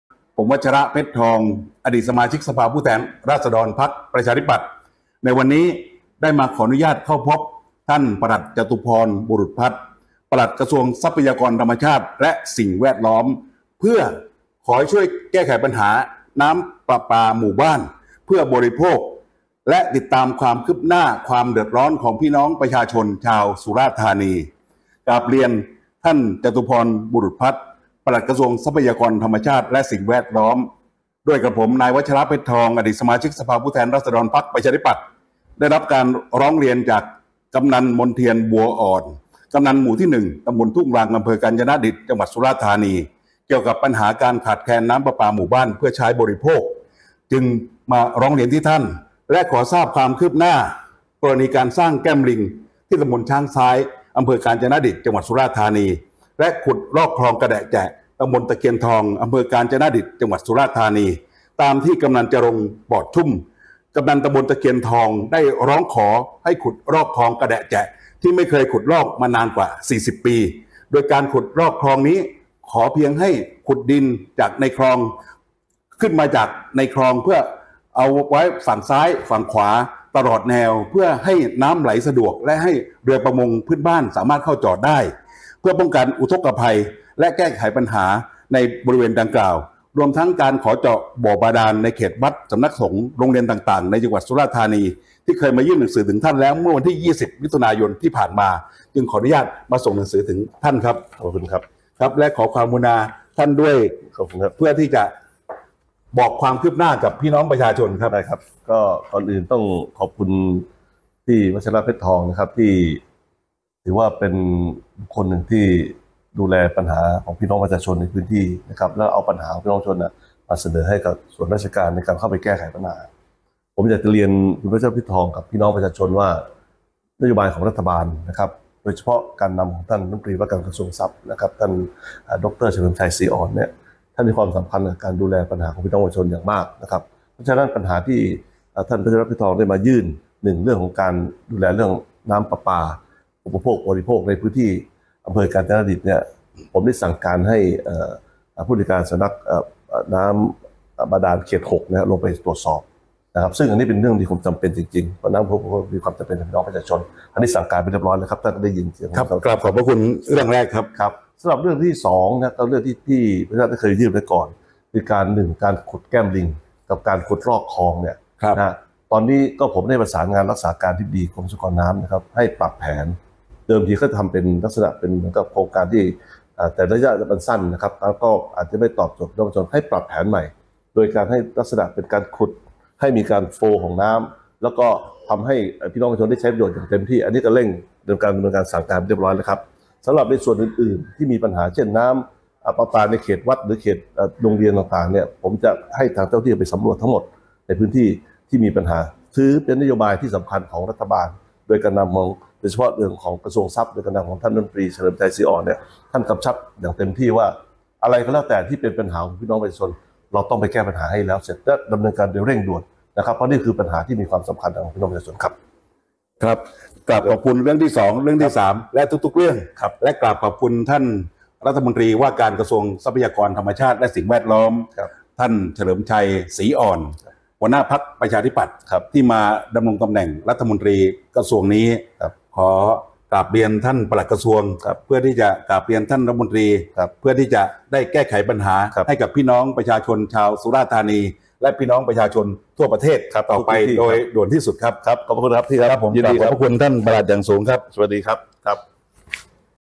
"วัชระ" บุกพบปลัดก.ทรัพย์ฯ จี้แก้ปัญหาชาวสุราษฎร์น้ำประปา-บาดาล ขุดลอกคลองกาญจนดิษฐ์ด่วน
อดีตสส.ปชป. เดินหน้าแก้ปัญหาให้พี่น้องชาวสุราษฎร์ จี้ปลัดก.ทรัพย์ฯ แก้ความเดือดร้อนของชาวบ้าน ทั้งปัญหาเก่าที่ตนเคยร้องและปัญหาใหม่ที่เพิ่งได้รับการร้องเรียน "ปลัดตุ๋ม" เผย ไม่นิ่งนอนใจ สั่งการให้ตรวจสอบแก้ไขโดยด่วนแล้วทั้ง 3 เรื่อง...